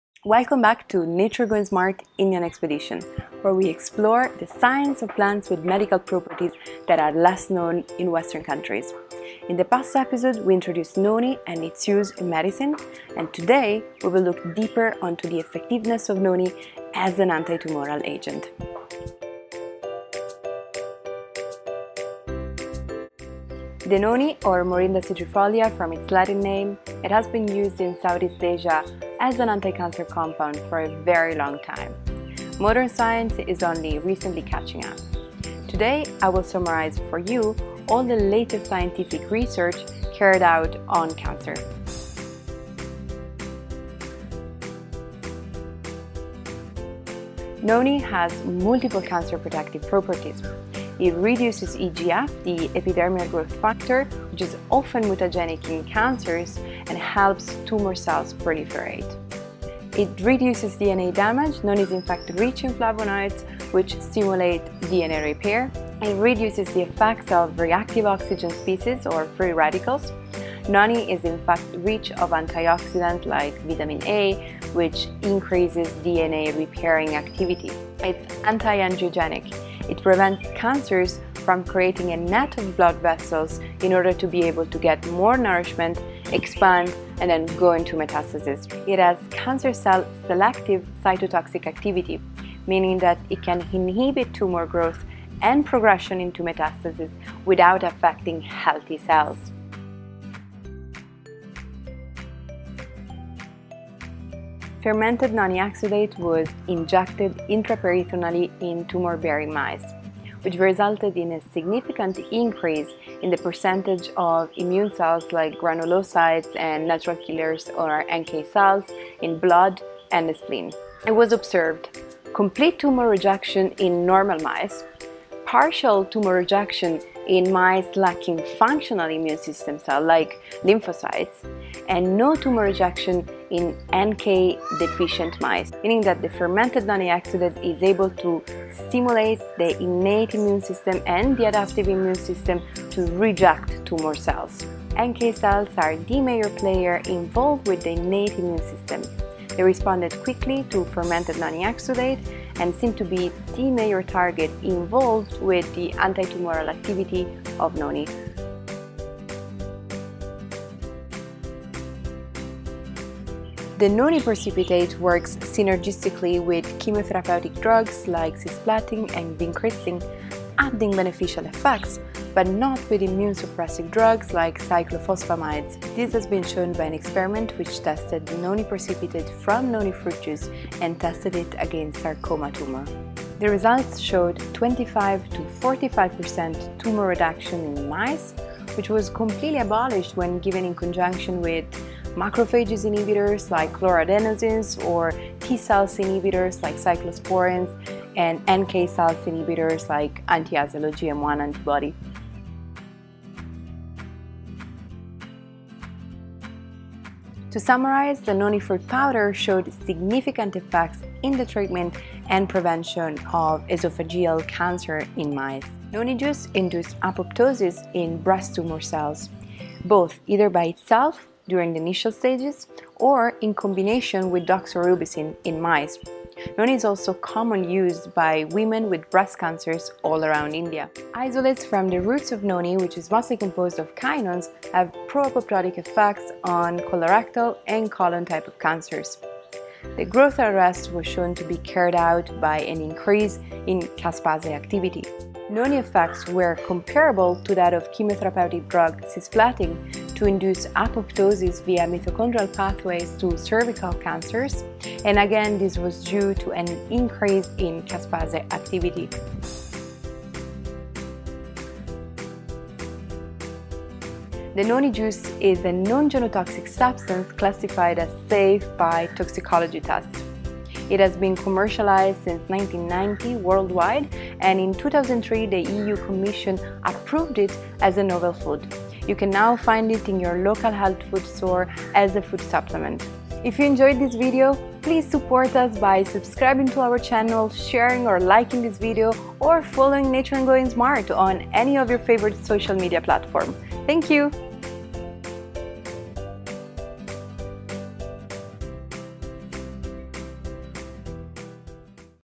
Noni vs Cancer : the Science
Welcome to the second of a series of science insights on plants with medical properties that have been used traditionally for hundreds of years.

You can watch the first chapter on Morinda Citrifolia or, as it is most commonly known, "Noni" plant

 We will review the scientific evidences which have been carried out in recent years to understand the traditional use of this plant and its mechanism of function for the treatment of various cancers.

References:

Liu, Guangming, et al. "Two novel glycosides from the fruits of Morinda citrifolia (noni) inhibit AP-1 transactivation and cell transformation in the mouse epidermal JB6 cell line." Cancer Research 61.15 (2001): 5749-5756.

Thani W, Vallisuta O, Siripong P, et al (2010). Anti-proliferative and antioxidative activities of Thai Noni/Yor (Morinda citrifolia Linn.) leaf extract. Southeast Asian J Trop Med Public Health, 41, 482-9.

Li J, Stickel SL, Bouton-Verville H, et al (2008). Fermented Noni Exudate (fNE): a mediator between immune system and antitumor activity. Oncol Reports, 20, 1505-9.

Furusawa E, Hirazumi A, Story S, et al (2003). Antitumour potential of a polysaccharide-rich substance from the fruit juice of Morinda citrifolia (Noni) on sarcoma 180 ascites tumour in mice. Phytother Res, 17, 1158-64.

Hornick CA, Myers A, Sadowska-Krowicka H, et al (2003). Inhibition of angiogenic initiation and disruption of newly established human vascular networks by juice from Morinda citrifolia (Noni). Angiogenesis, 6, 143-9.

Stoner GD, Wang LS, Seguin C, et al (2010). Multiple berry types 50.0 prevent N-nitrosomethylbenzylamine-induced esophageal cancer in rats. Pharm Res, 27, 1138-45.

Taşkin EI, Akgün-Dar K, Kapucu A, et al (2009). Apoptosis-inducing effects of Morinda citrifolia L. and doxorubicin on the Ehrlich ascites tumor in Balb-c mice. Cell Biochem 0 Funct, 27, 542-6.

Lv L, Chen H, Ho CT, et al (2011). Chemical components of the roots of Noni (Morinda citrifolia) and their cytotoxic effects. Fitoterapia, 82, 704-8.

Gupta RK, Banerjee A, Pathak S, et al (2013). Induction of Mitochondrial-Mediated Apoptosis by Morinda citrifolia (Noni) in Human Cervical Cancer Cells. Asian Pac J Cancer Prev, 14, 237